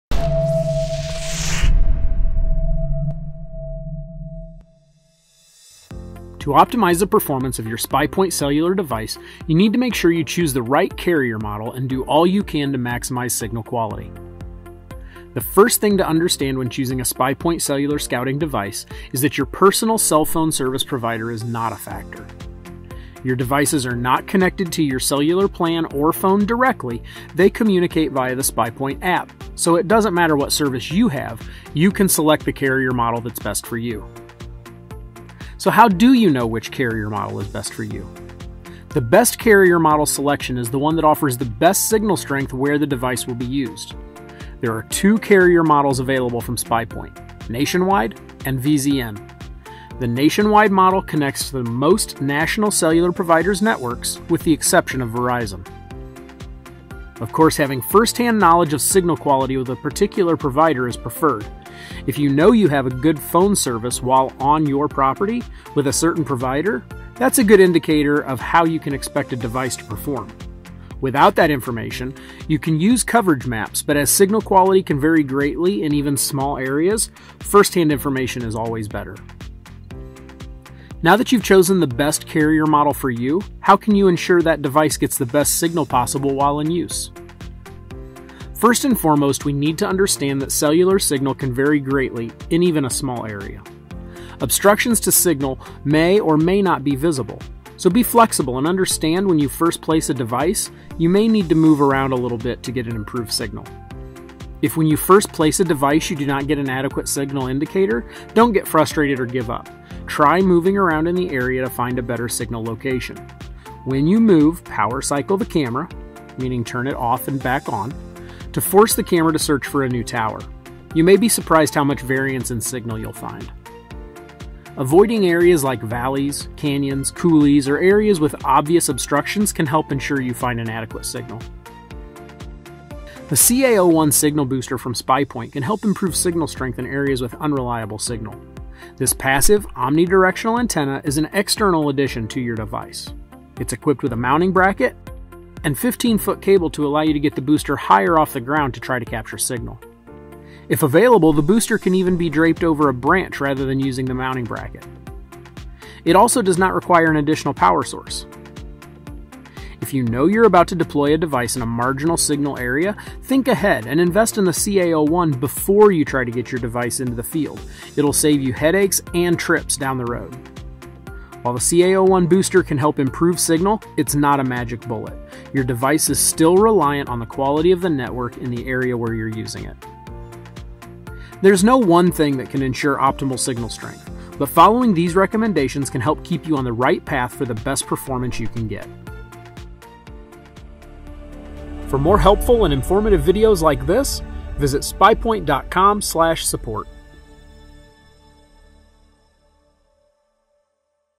How to choose the right carrier model and maximize signal strength
Choosing the right carrier model for your SPYPOINT mobile scouting device can ensure you have the best performance and signal quality.

SPYPOINT mobile scouting devices are available in two carrier models, so choosing the right one is important so you can get the best performance possible. The main thing to keep in mind when choosing carrier model is that your personal cellular service provider is not a factor. Choose the carrier model that offers the most reliable signal where the device will be used. That’s it, it’s that simple.

Because SPYPOINT photo transmission plans are purchased directly from SPYPOINT, not through cellular providers, the process is quick and easy. Not only that, you aren’t tied to one provider that may not have reliable signal in every area you want to use a SPYPOINT device. You can have any mix of carrier models in your SPYPOINT account, so you never have to wish you had a different option.

Once the carrier model is selected, make sure you’re doing all you can to ensure a reliable signal during the installation procedure. Following these simple guidelines can save you a lot of headaches down the road from poor planning.

0:15 – The main thing to understand
0:34 – Which carrier model is best for you?
0:56 – How to gauge signal quality
1:20 – How to get the best signal quality when installing
2:04 – Areas to avoid
2:13 – Tools to improve signal
2:39 – Plan ahead for success
2:50 – Network reliability